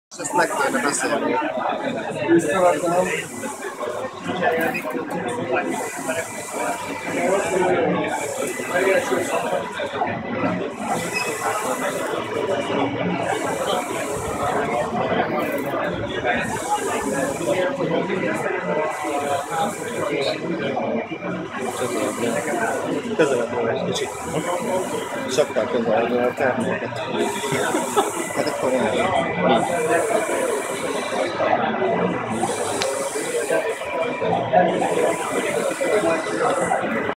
3D printing the LED display
We were attending to the opening of the 3D printing showroom of Varinex Zrt. who is the largest 3D printing company of Hungary and were pleasantly surprised to see that one of the printers, namely a venerable Objet Connex 500 was printing 3 LED displays of our prototypes. Watching these printers in action is always very interesting so here's a short video for your viewing pleasure.